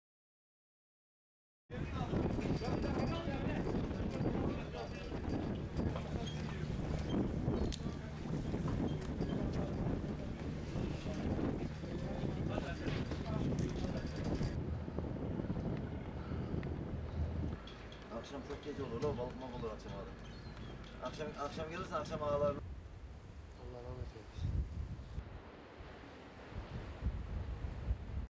Turquie: une embarcation de migrants chavire, au moins 24 morts
Au moins vingt-quatre personnes sont mortes lundi matin, 3 novembre, dans la partie nord du détroit du Bosphore, à proximité de la mer Noire, lors du chavirage d'un bateau qui transportait une quarantaine de migrants, ont rapporté les médias turcs. Durée: 00:28